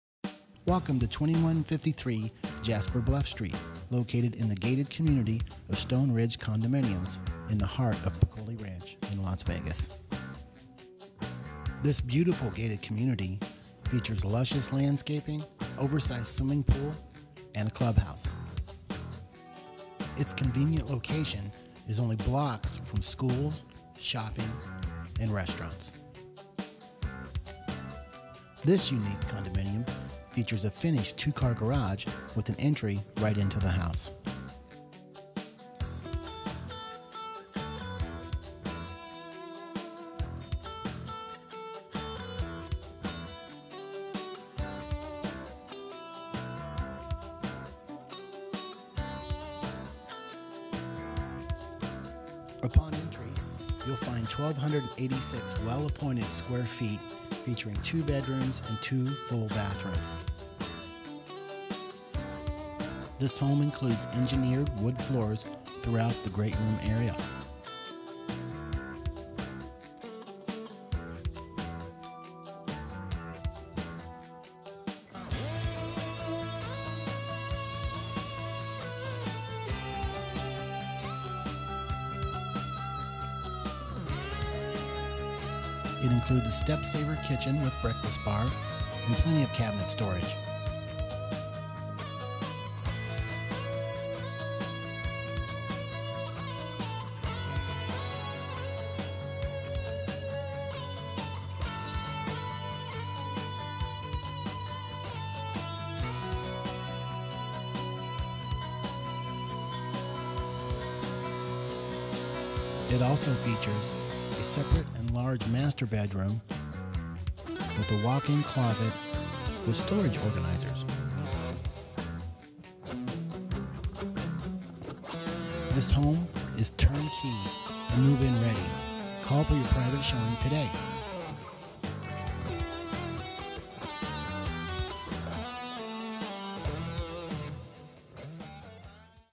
Las Vegas Condo For Sale
Beautiful Las Vegas condo for sale located in a gated community in Pecolle Ranch. Features an attached 2 car garage!  Super nice and move in ready.